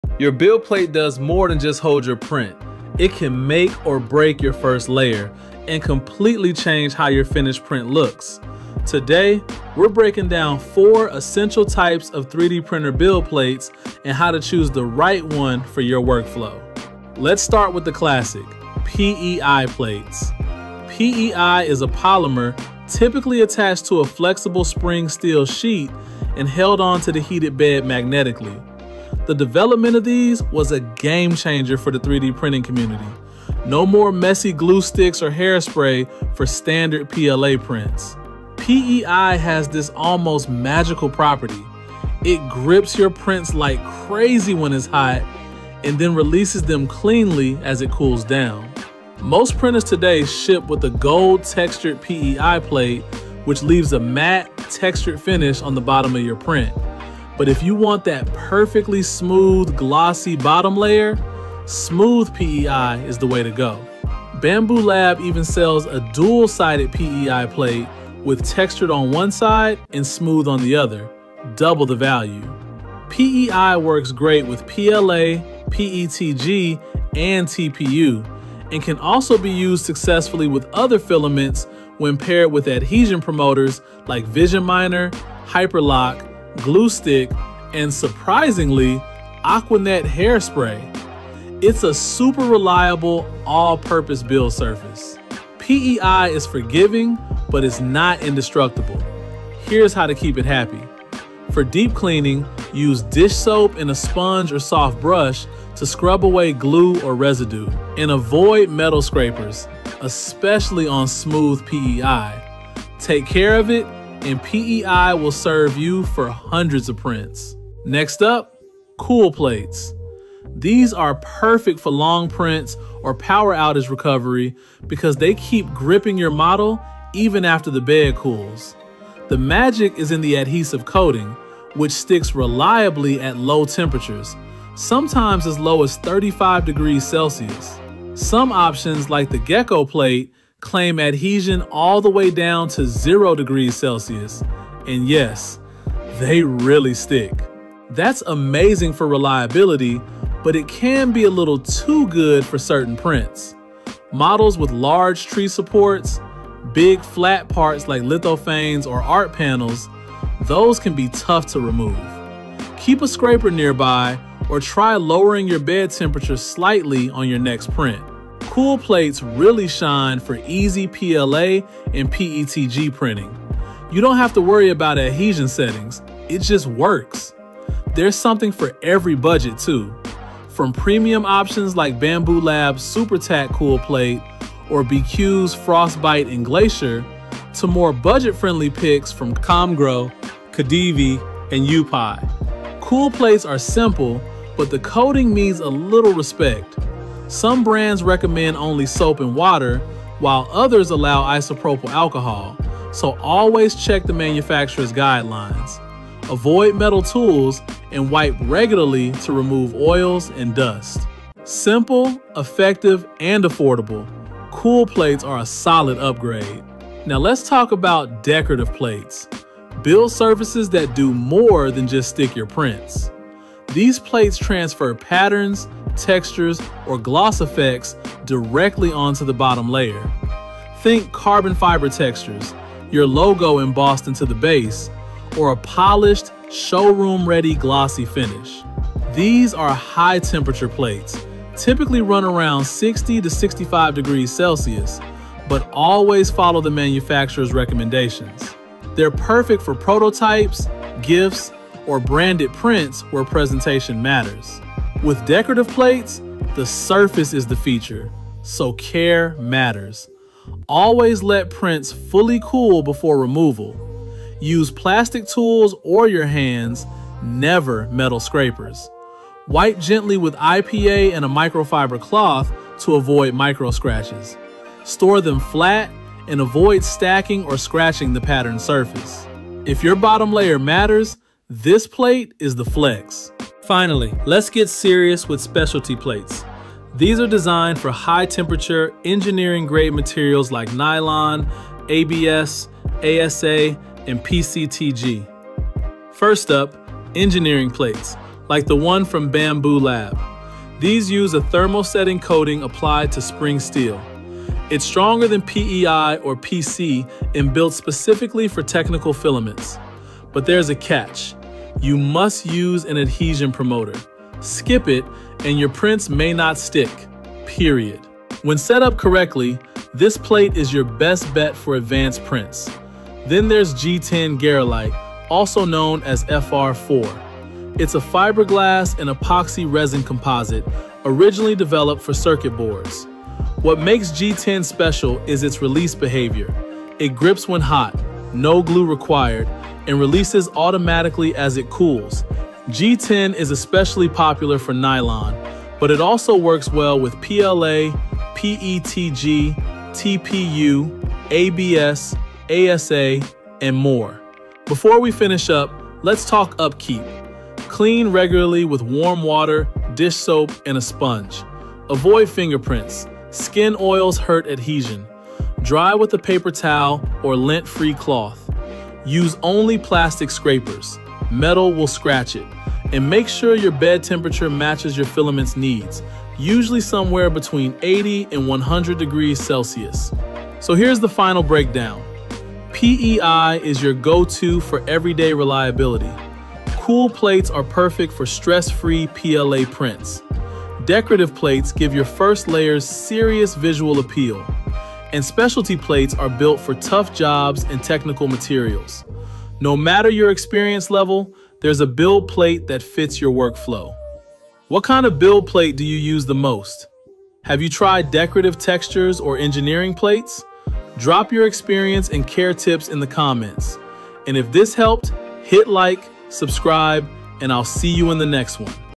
Best 3D Printer Build Plates Explained
Your 3D printer build plate does more than hold your print — it can make or break your first layer and completely change how your finished print looks.

In this video, I break down the four most important types of 3D printer build plates and explain which one you should actually be using based on your materials and workflow.

Build Plates Covered:

PEI Build Plates (textured & smooth)
Cool Plates (SuperTack, Frostbite, Glacier, and more)
Decorative Build Plates (textures, patterns, glossy finishes)
Specialty & Engineering Plates (Bambu Engineering Plate & G10 Garolite)

Whether you’re running a Bambu Lab printer or any other FDM machine, this guide will help you choose the best build surface for reliable, stress-free prints.

👇 Drop a comment and let me know which build plate you use most — or if you’ve tried decorative or engineering plates.

Products Featured
PEI Plates
Biqu Dual Textured and Smooth

Biqu Frostbite
Biqu Glacier
Juupine Geeco

IdeaFormer Decorative Plates

Adhesion Promoters
Glue Stick
Bambu Liquid Glue
Aqua Net Hairspray
Vision Miner